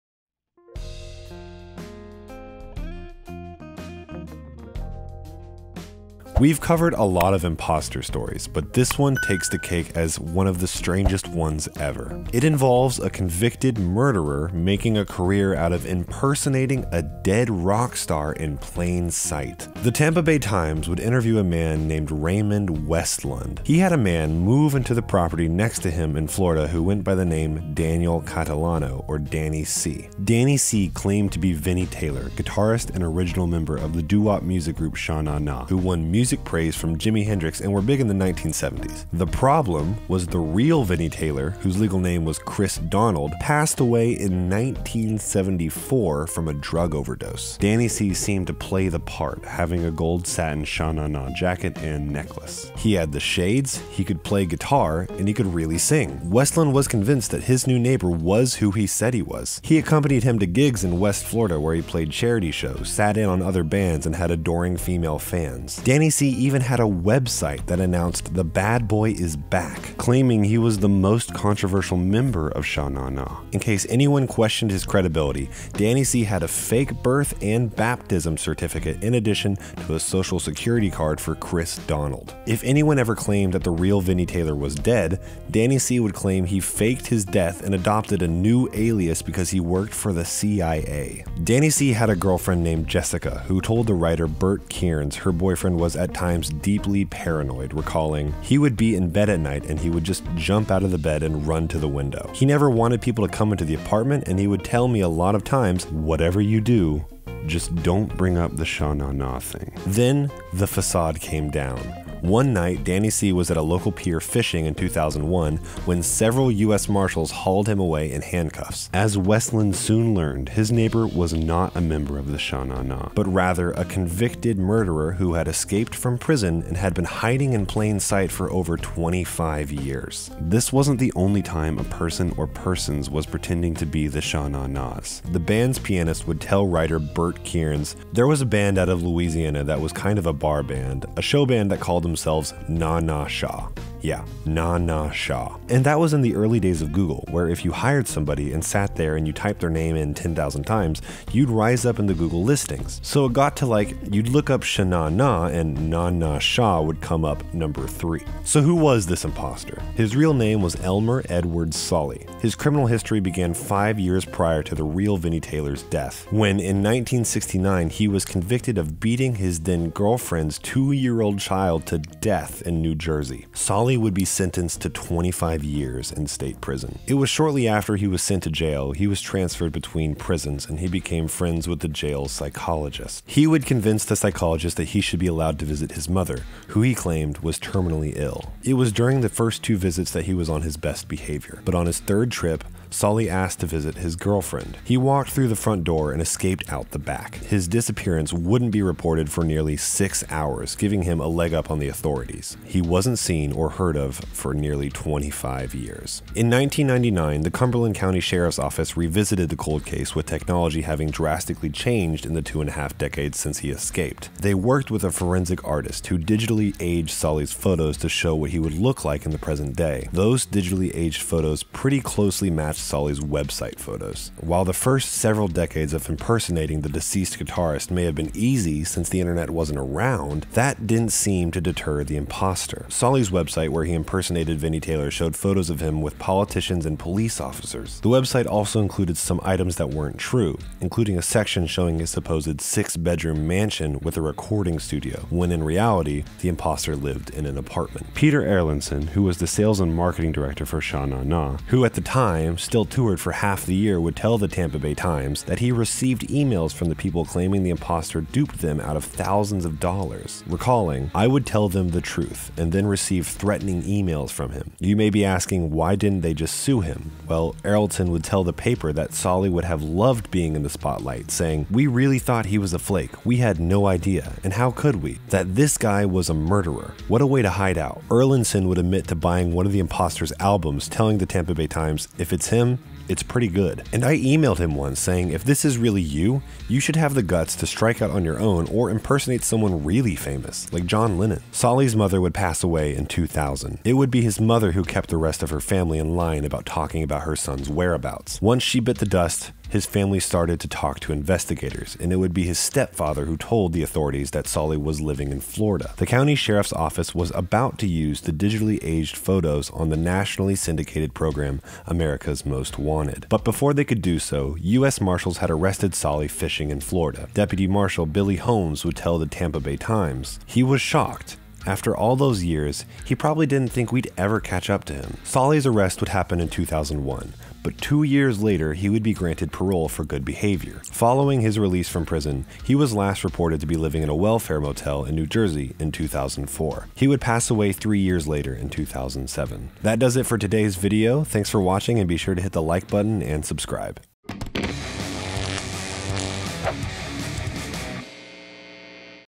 Sha Na Na: The Vinnie Taylor Imposter Who Was a Murderer
The Vinnie Taylor Imposter Who Was a Murderer

We’ve  covered quite a few imposter stories, but this one takes the cake as one of the strangest one’s ever. It involves a convicted murderer making a career out of impersonating a dead rockstar in plain sight. That’s what were going to explore in todays video.

The Tampa Bay Times would interview a man named Raymond Westlund. He had a new man move into the property next to him in Florida who went by the name Daniel Catalano, or Danny C. Danny C  claimed to be Vinnie Taylor, guitarist and original member of the doo-wop music group Sha Na Na who won much praise from Jimi Hendrix and were big in the 70’s.  The problem was the real Vinnie Taylor whose real name was Chris Donald, passed away in 1974 due to a drug overdose.

Danny C played the part having a gold satin Sha Na Na jacket and necklace.  He had the shades, could play guitar and he could really sing. Westlund was convinced that his new neighbour was who he said he was since he accompanied him to gigs in west florida where he played charity shows, sat in with other bands and had adoring female fans.

Danny C even had a website that announced “The Bad Boy is back!” claiming he was the most controversial member of Sha Na Na. Incase anyone questioned his credibility, Danny C had a fake birth  and baptism certificate in addition to a social security card for Chris Donald. If anyone was aware that the real Vinnie Taylor was dead, Danny C would claim he faked his death and adopted a new alias because he worked for the CIA.

Danny C had a girlfriend named Jessica who told the writer Burt Kearns her boyfriend was at times deeply paranoid recalling “We would be in bed at night and he would just jump out of bed and run to the window, He never wanted people coming into our apartment, and he would tell me a lot of times, ‘Whatever you do, just don’t bring up the Sha Na Na thing’.”

Then, the facade came down. One night as danny c was on a local pier fishing in 2001 several US Marshalls hauled him away in handcuffs. As Westlund soon learned his neighbour was not a member of the Sha Na Na, but rather a convicted murderer who escaped from prison and had been hiding in plain sight for over 25 years, albeit under a different alias.

This wasn’t the only time a person or persons were pretending to the be the sha na na na’s happened. The band’s pianist would tell writer Burt Kearns “There was a band out of Louisiana that was kind of a bar band, a show band that called themselves Na Na Sha. Yeah.  ‘Na Na Sha.'”  “And that was in the early days of Google, where if you hired somebody and sat there and typed in your name ten thousand times, you’d rise up in the Google listings. So it got to like, when you’d look up Sha Na Na, ‘Na Na Sha’ would come up number three.”

So who was the imposter? Well his real name was Elmer Edward Solly. His criminal history began five years prior to the real Vinnie Taylor dying, when in 1969 when he was convicted of beating to death his then girlfriend’s 2 year old child in New Jersey. Solly would be sentenced to 25 years in state prison.

It was shortly after he was sent to jail, he was transferred between prisons and he became friends with the jail’s psychologist. He would convince the psychologist that he should be allowed to visit his mother who he claimed was terminally ill. It was during the first two visits that he was on his best behavior, but ib his third trip, Solly asked to visit his girlfriend. He walked through the front door and out the back escaping. His disappearance wouldn’t be reported for nearly 6 hours giving him a sufficient leg up on the authorities. He wasn’t seen or heard from again for nearly 25 years.

In 1999 the Cumberland Country Sheriff’s office revisited the cold case and with technology having drastically changed in the two and a half decades since he escaped, they worked with a forensic artist who digitally aged Solly’s photos to show what he would look like in present day. Those digitally aged photos m